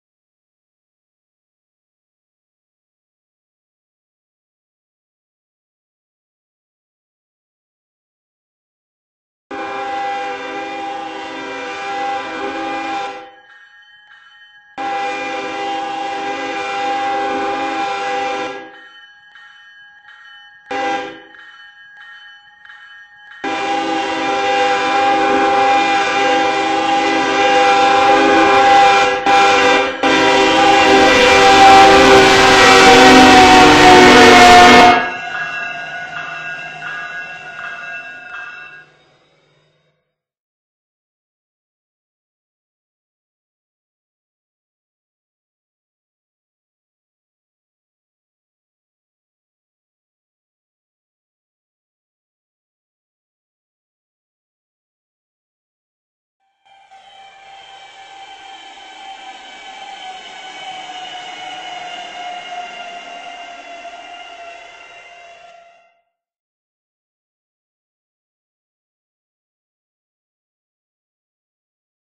High Speed UP C45ACCTE 7640 Intermodal Train At Grand Island, NE Railcam
DPU UP C44AC 6423
Intermodal Train